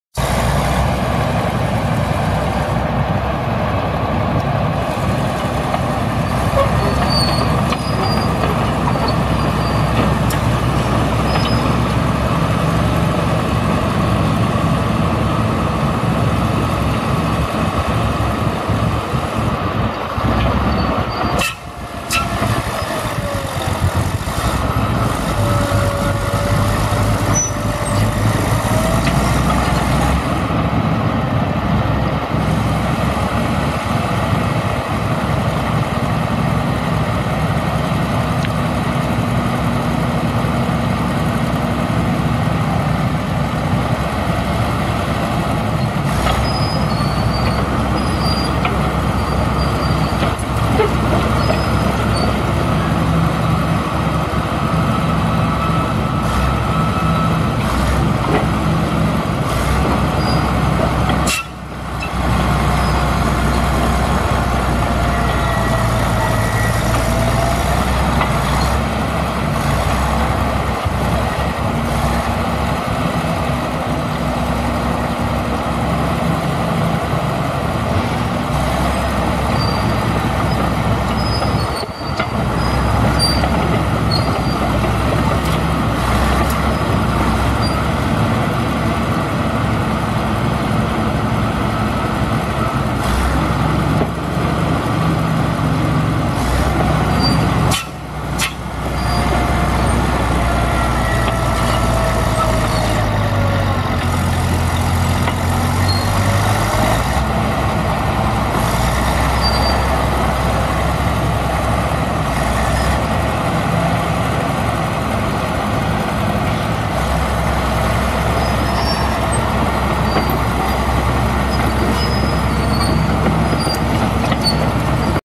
Caterpillar D6 pushing sand at Zagray Farm
Pushing sand with a root rake!